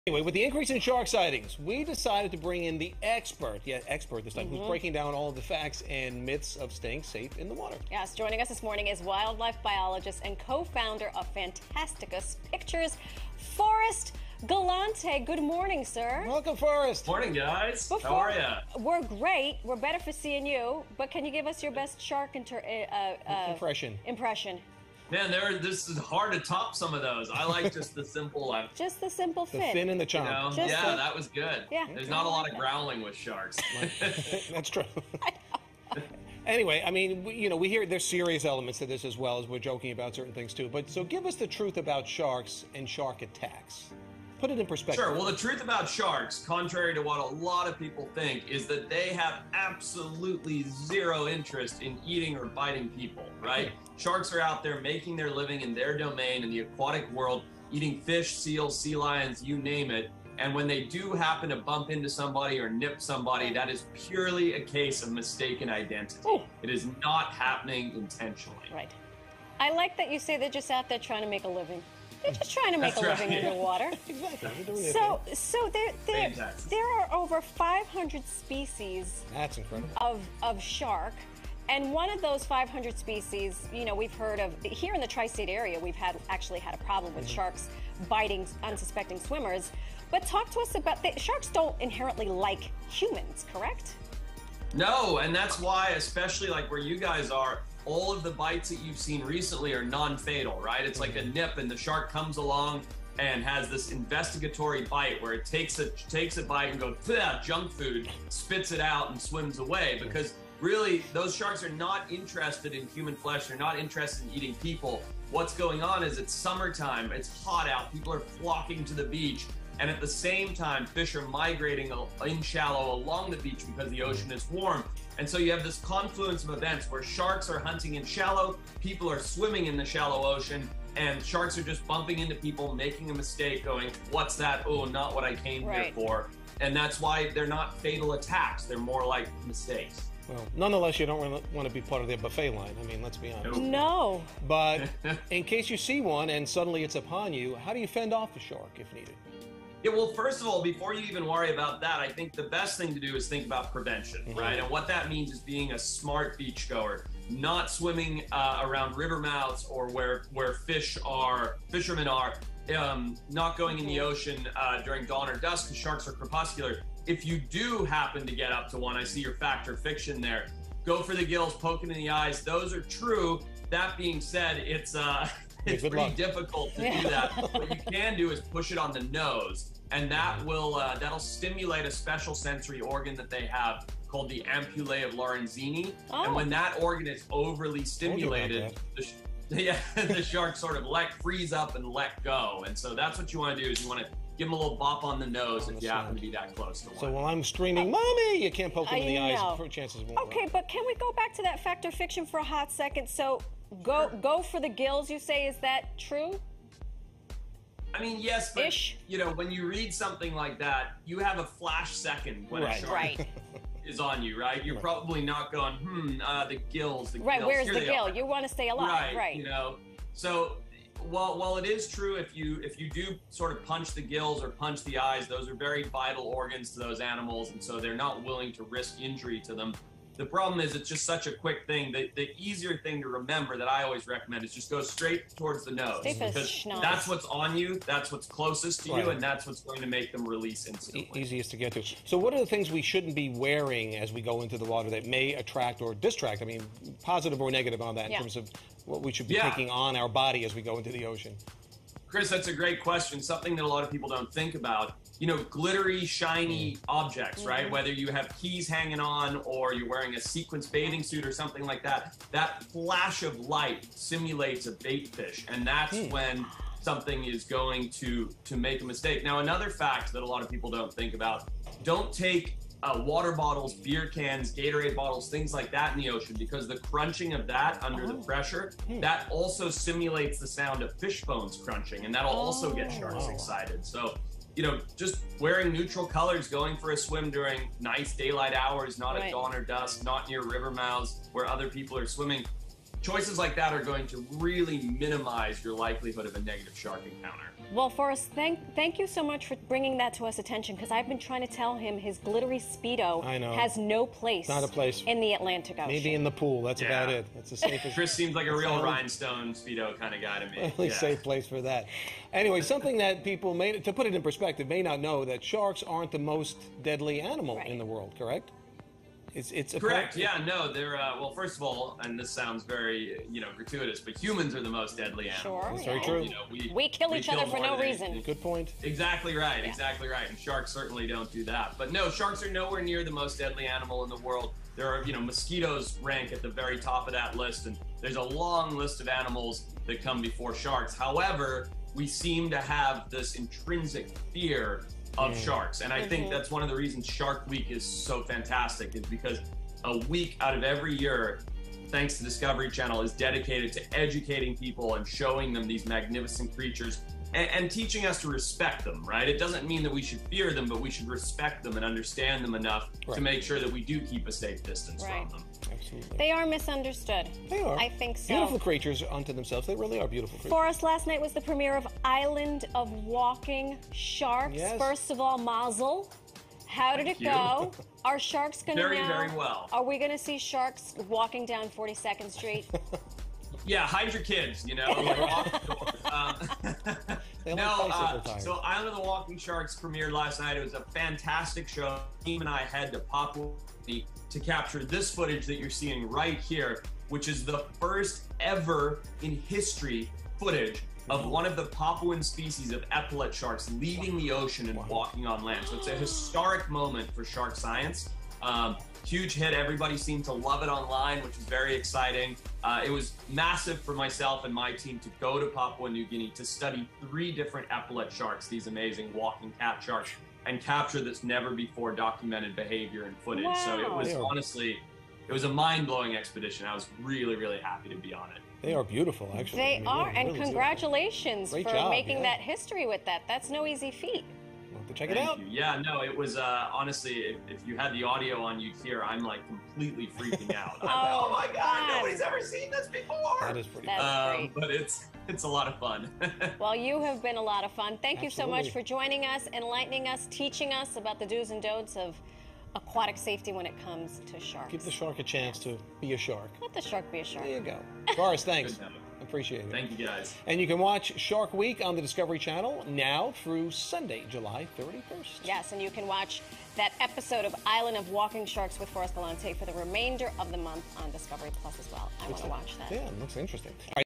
Shark Week: Debunking myths with Forrest Galante
Forrest Galante, wildlife biologist and show host, joined New York Living to bust myths and tell facts about some of the most misunderstood marine creatures --- sharks.